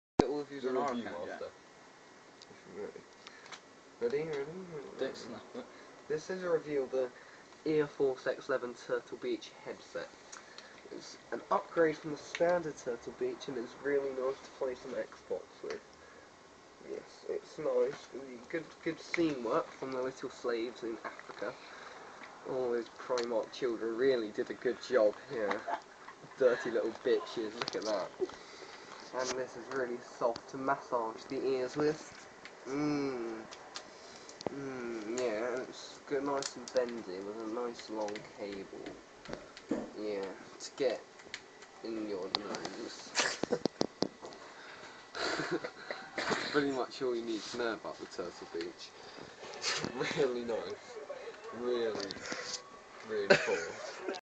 Turtle Beach Review
this is a expert review on a turtle beach headset for the xbox 360